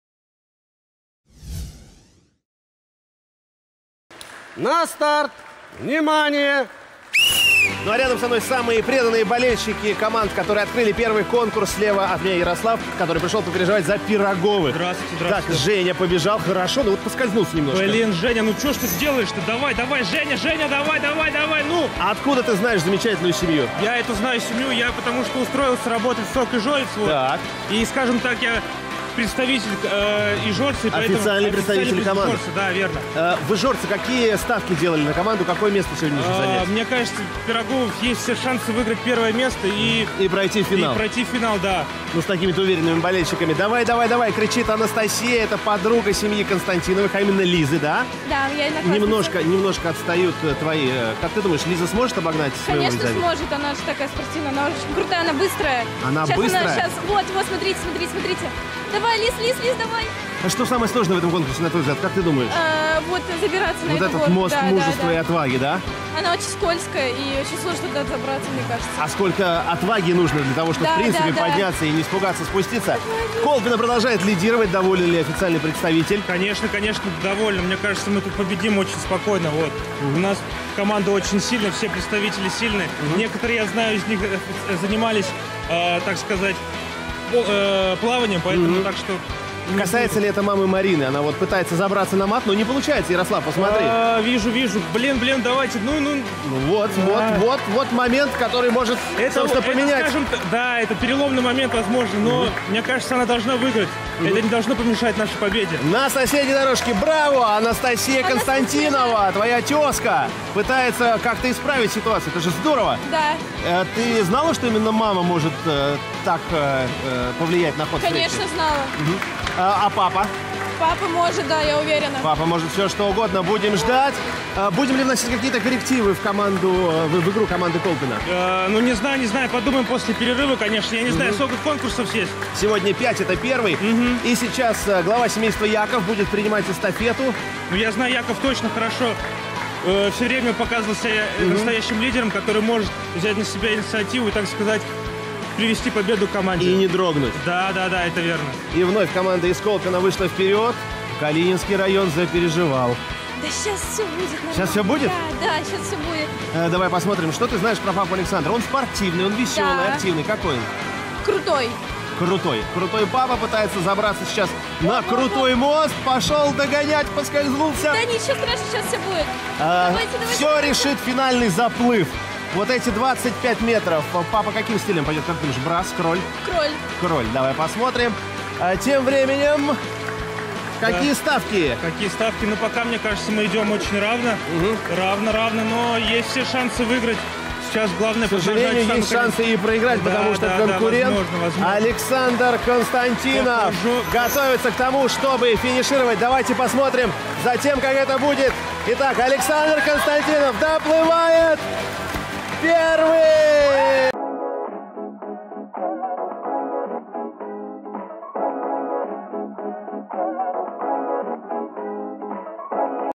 Надувная полоса препятствий. Водная горка
В видеоролике представлены надувные аттракционы TimeTrial, которые можно приобрести на сайте

Надувной аттракцион "Полоса препятствий" от TimeTrial идеально подходит для проведения этапов командных соревнований на любом тимбилдинге или спортивном празднике.

"Полоса препятствий" придаст любому конкурсу динамики и зрелищности. Гости мероприятия с азартом устремятся к преодолению преград!